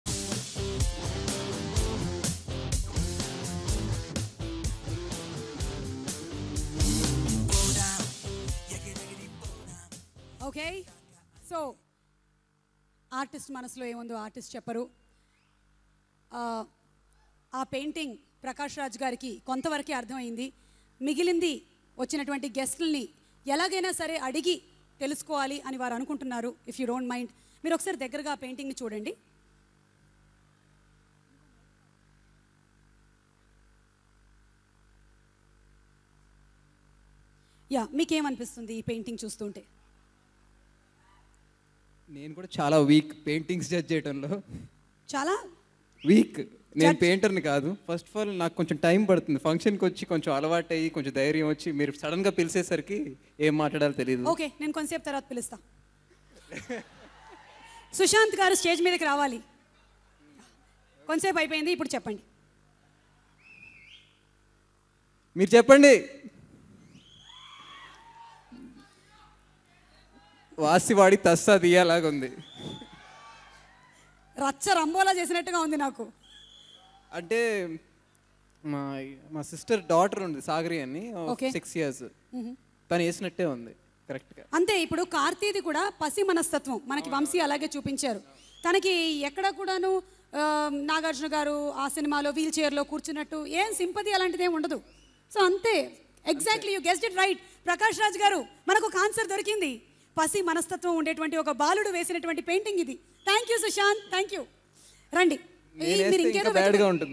Sushanth Funny Comments on Karthi's Painting @ Oopiri Movie Thank You Meet
Watch Oopiri Movie Thanks Meet. Oopiri is a family drama acted by King Nagarjuna, Karthi, Thamanna, Directed by Vamsi Paidipally and Music by Gopi Sundar.